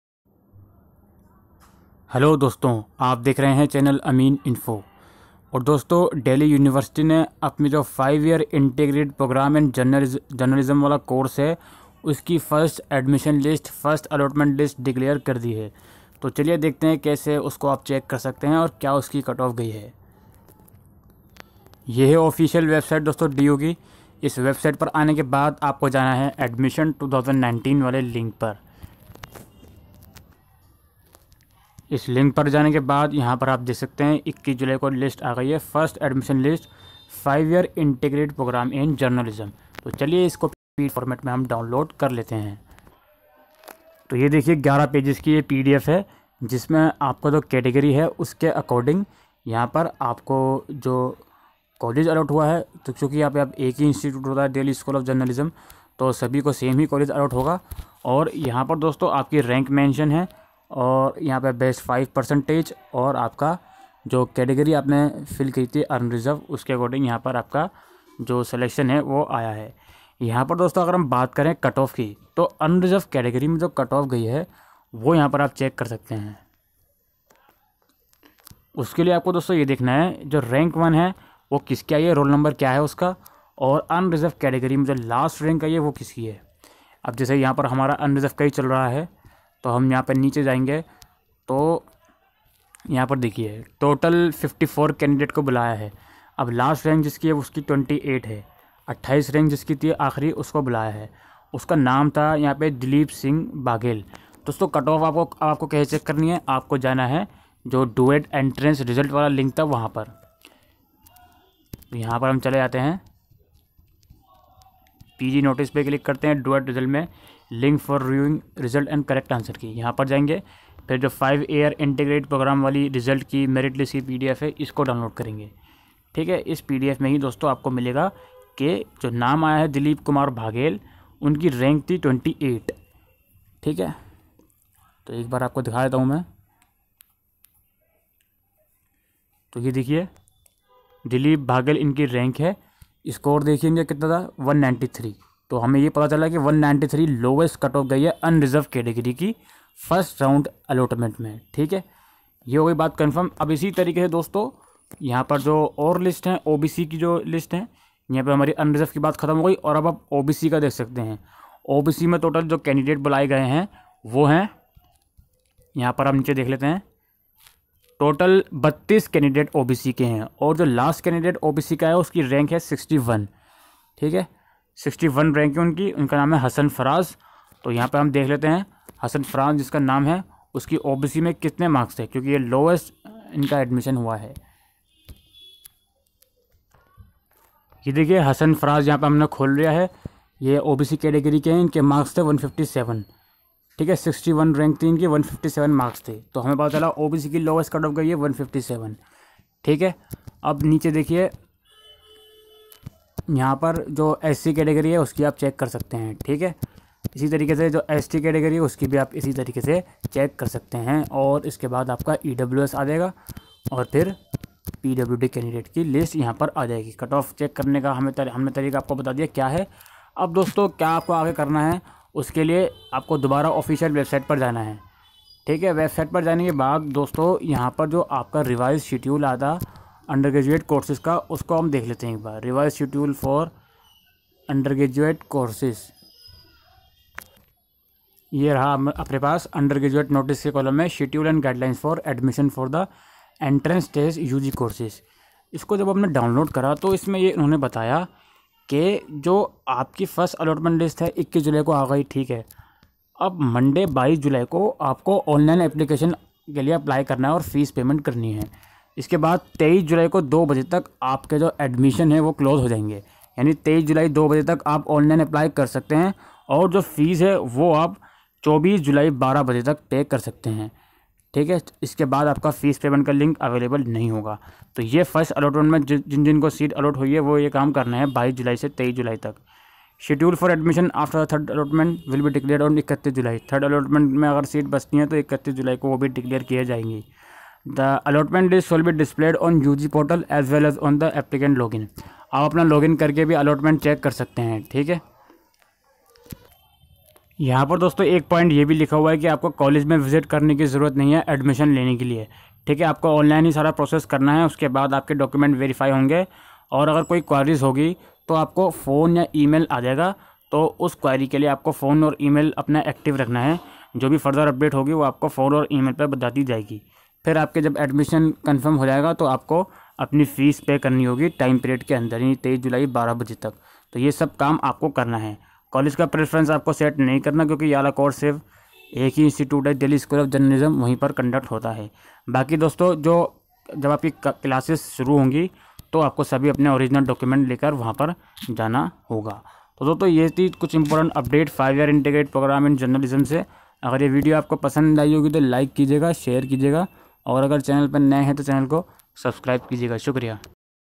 DELHI UNIVERSITY UG FIRST ADMISSION LIST 2019!! DELHI UNIVERSITY ENTRANCE EXAM RESULT 2019
Hii everyone
This video provided information related to
Delhi University five years course in journalism first admission list 2019